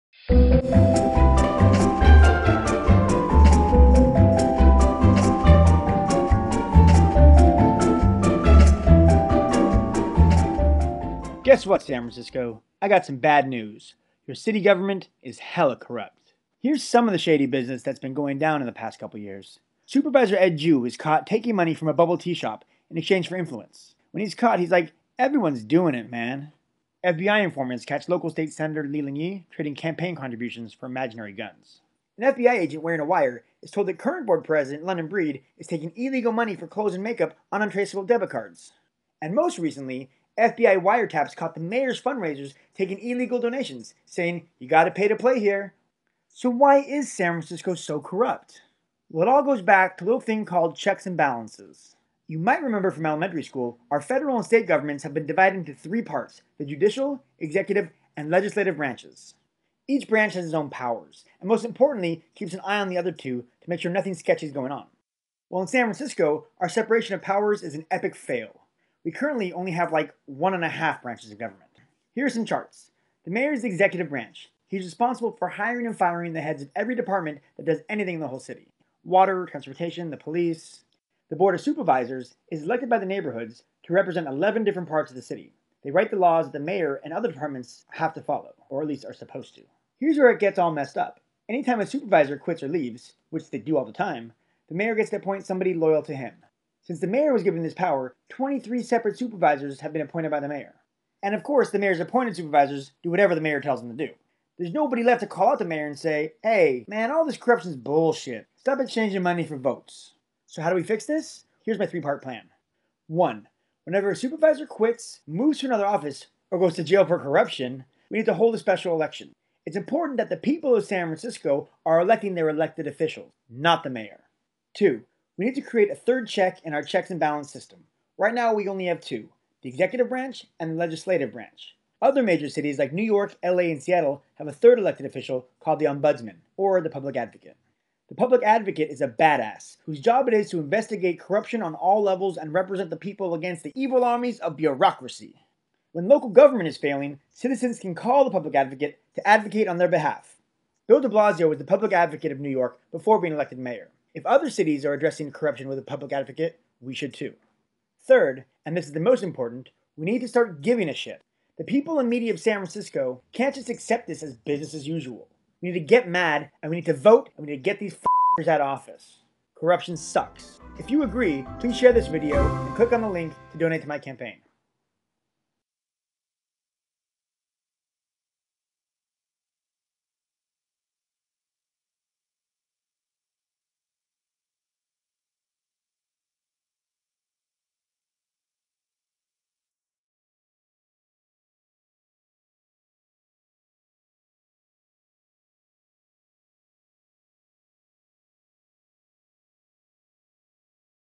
Solving San Francisco Political Corruption by Broke Ass Stuart
How to Tackle San Francisco’s Political Corruption Problem By GoForBroke Broke-Ass Stuart

He has his own YouTube channel where he posts other super informative SF-centric videos

To support the burgeoning Broke-Ass campaign for mayor of San Francisco Go